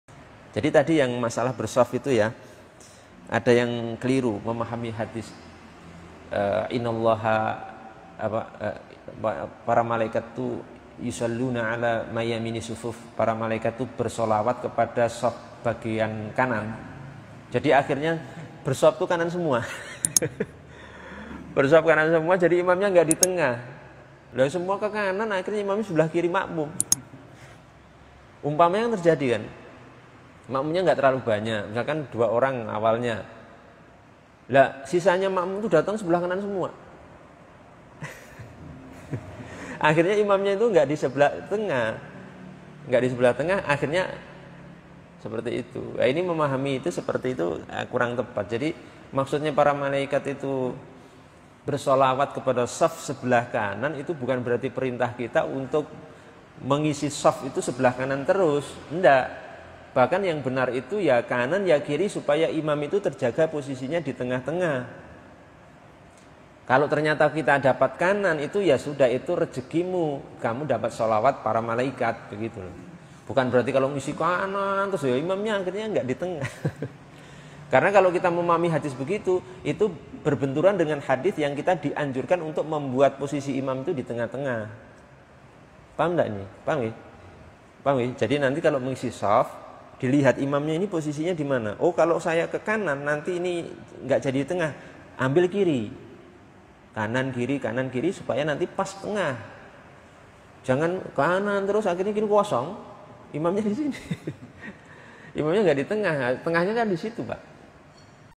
MENGISI SHAF MULAI SEBELAH KANAN ??, Ust. Abdullah Al-Jirani
Ada yang memahami mengisi shaf sebelah kanan dulu. Sehingga kadang-kadang terjadi suatu kondisi di mana imamnya tidak berada di tengah. Bagaimana sebenarnya masalah ini ?